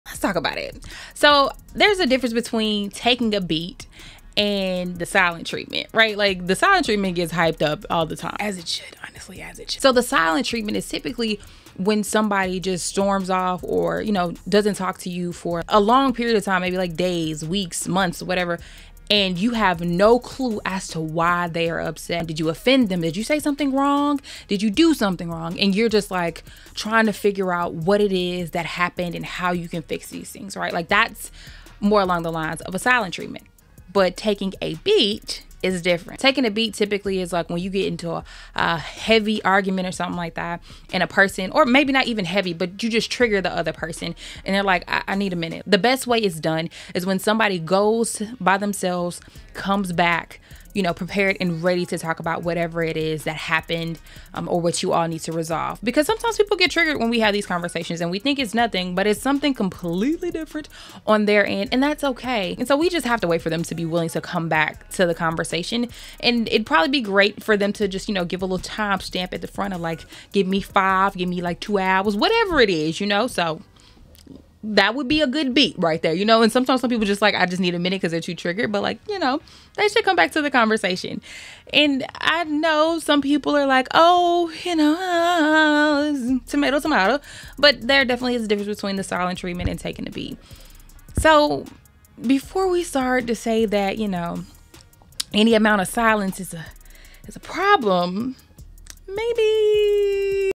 Talking TikTok-Learning Boundaries: Taking a beat vs The silent Treatment
This video is about boundaries. Particular those from taking a minute when you're triggered or overwhelmed.